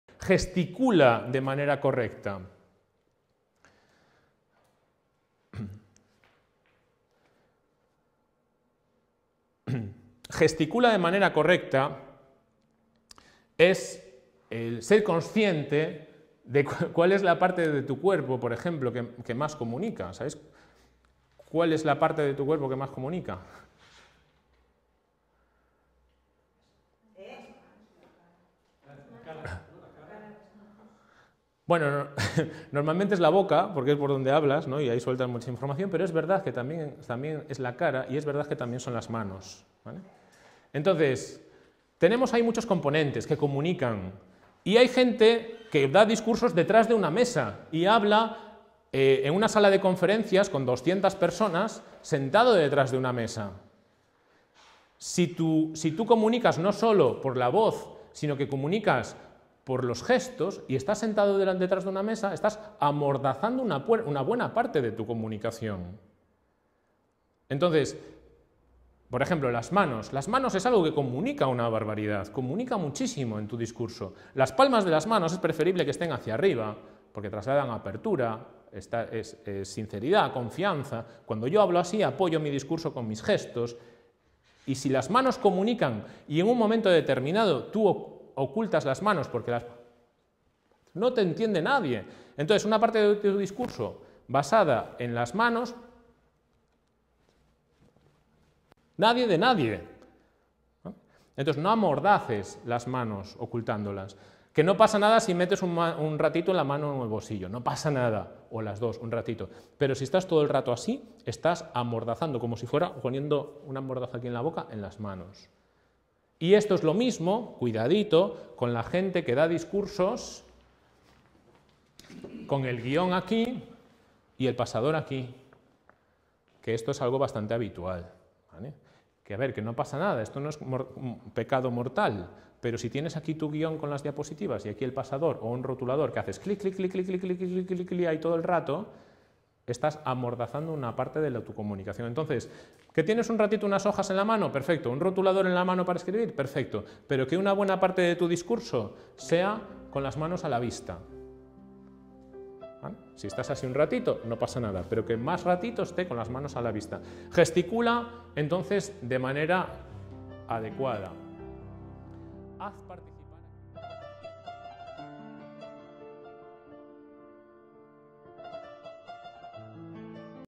Los mejores GESTOS para hablar en público
Domina el lenguaje del cuerpo para hacer exposiciones en público exitosas